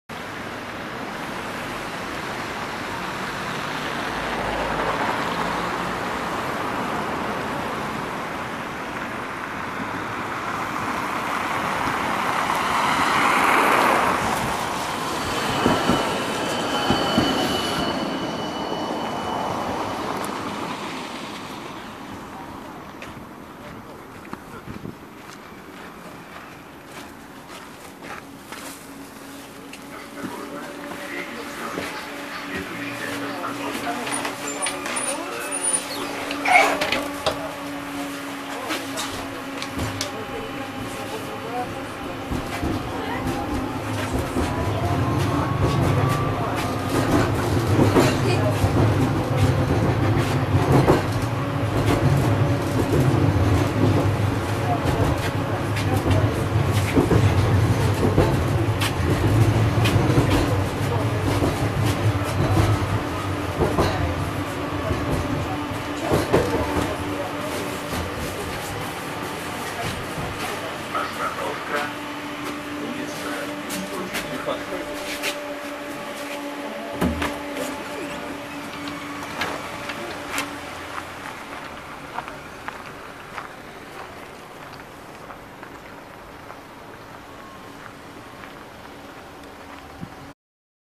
Трамвай Санкт-Петербурга 8-37: 71-923 "Богатырь" б.0203 по №100 (07.01.18)
Трамвай 71-923 "Богатырь", бортовой 0203, следующий по маршруту №100.
Видео снято на участке: проспект Просвещения (от ост. "ул. Руднева" до ост. "ул. Кустодиева")
Дата съемки: 7 января, 2018 в 17:44.